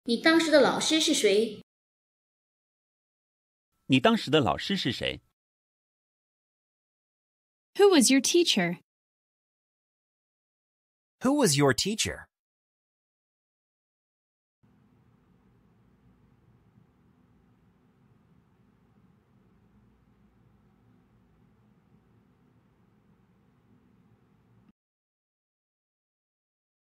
How to say "Who was your teacher?" in Chinese (Traditional)
Male Voice:
你當時的老師是誰？

Male Voice Translation:
Who was your teacher?